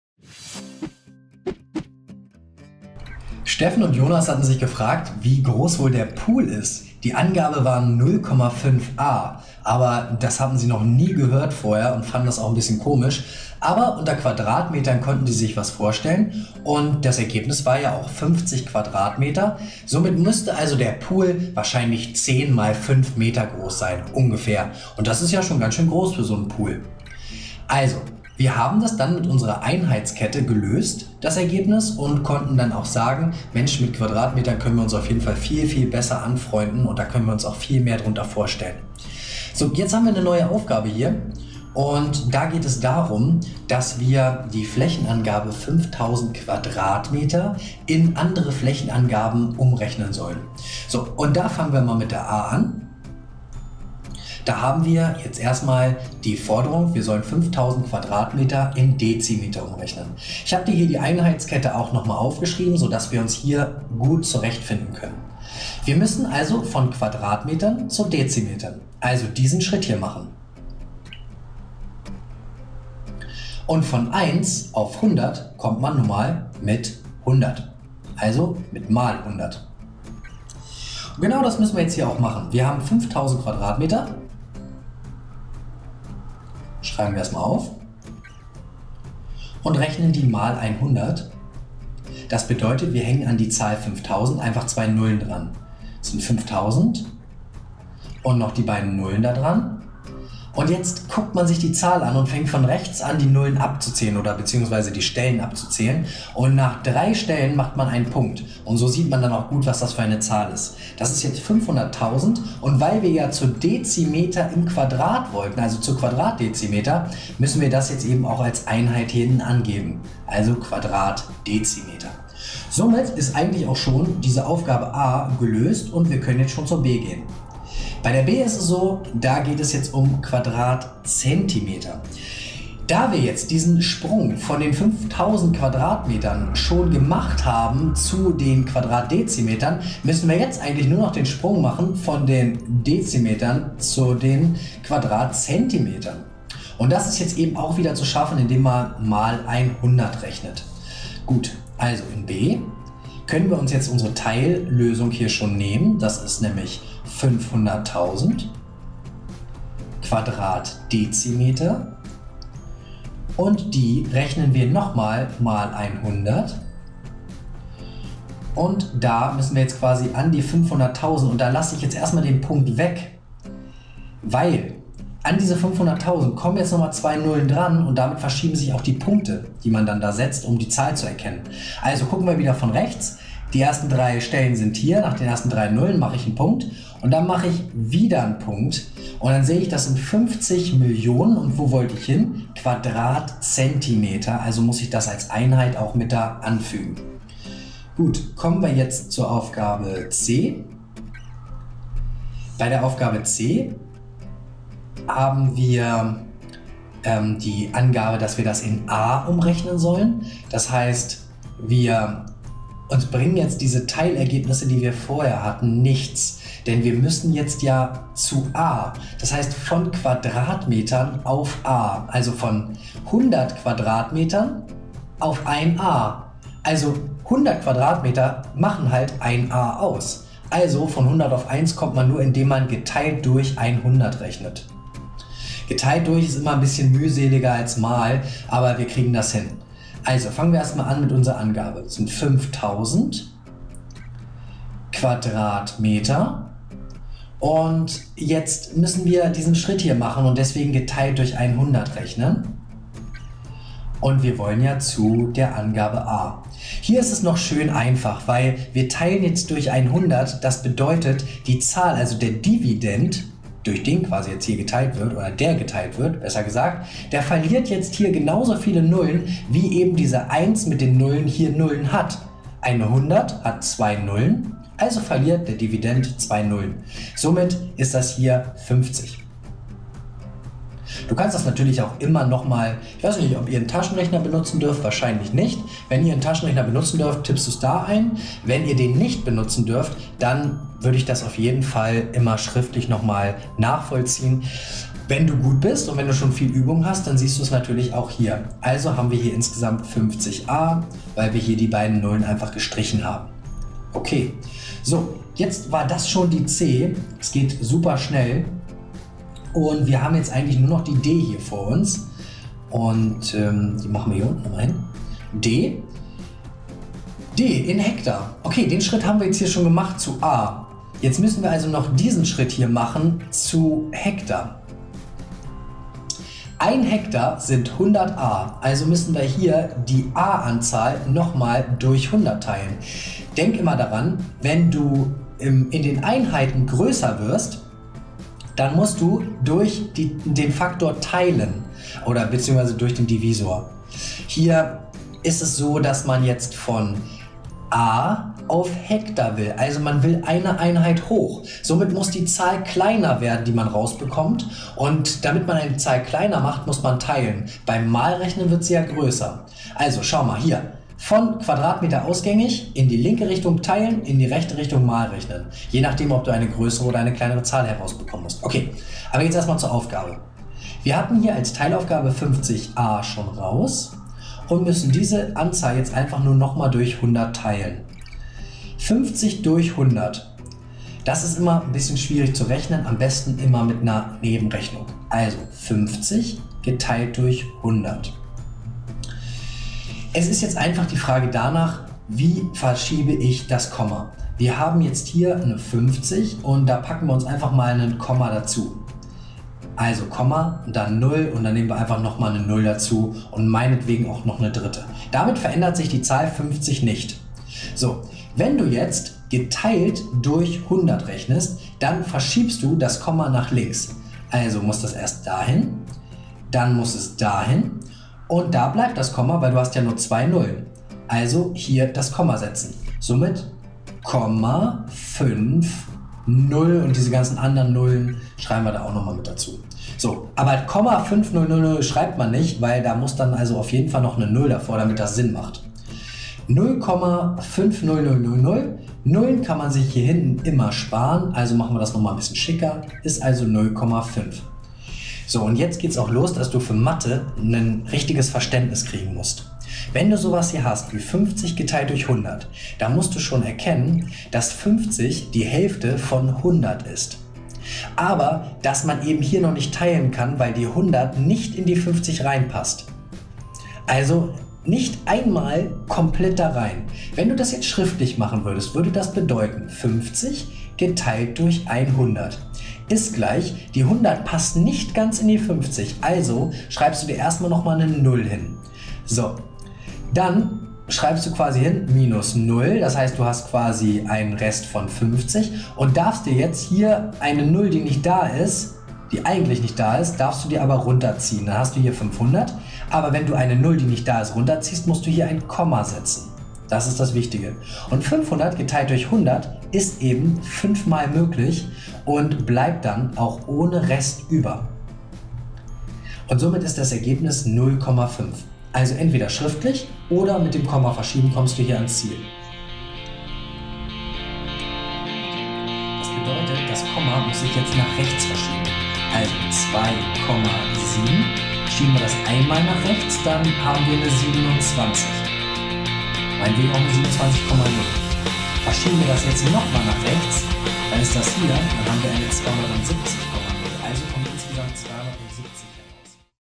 Flächen umrechnen - Quadratmeter || Klasse 5 ★ Übung 1
Das zugehörige StrandMathe-Übungsheft mit QR-Codes zu Lösungsvideos für alle Aufgaben erhältlich

Auch bei der Angabe von Flächen gibt es verschiedene Größenangaben, die sich ineinander umrechnen lassen. Dabei sind die Umrechnungsfaktoren einheitlich. Die üblichen Größen sind: Quadratkilometer km^2, Hektar ha, Ar a, Quadratmeter m^2, Quadratdezimeter dm^2, Quadratzentimeter cm^2, Quadratmillimeter mm^2

Steffen: „Gestern hat jemand erzählt, dass unser Pool 0,5 Ar groß sein soll, ich weiß aber gar nicht was das für eine Einheit ist.“
Jonas: „Ich glaube das ist eine andere Angabe für Quadratmeter, lass uns mal versuchen das umzurechnen.“

Bei der Umrechnung von Flächen gibt es den einheitlichen Umrechnungsfaktor 100. Der Größe nach sind die Flächenangaben wie folgt sortiert:
 Quadratkilometer - Hektar - Ar - Quadratmeter - Quadratdezimeter - Quadratzentimeter - Quadratmillimeter
 1 km^2=100 ha→1 ha=100 a→1 a=100 m^2→1 m^2=100 dm^2→1 dm^2=100 cm^2→1 cm^2=100 mm^2
 Die Umrechnung von 0,5 Ar in Quadratmeter kannst du daher durch Multiplikation mit 100 bestimmen. Der Zahlenwert muss größer werden, da ein Ar eine größere Fläche darstellt als Quadratmeter.
 0,5 a∙100=50 m^2
 Die Bestimmung aller anderen Werte kannst du genauso durchführen.
 50 m^2∙100=5000 dm^2
 500 dm^2∙100=500000 cm^2
 500000 cm^2∙100=50000000 mm^2
 0,5 a∶100=0,005 ha
 0,005 ha∶100=0,00005 km^2
Tipp: Wie bei allen anderen Umrechnungen überlegst du dir vorher am besten, ob die Zahl größer oder kleiner werden muss.

Steffen: „Also ist unser Pool 50 Quadratmeter groß. Unter der Angabe kann ich mir etwas vorstellen.“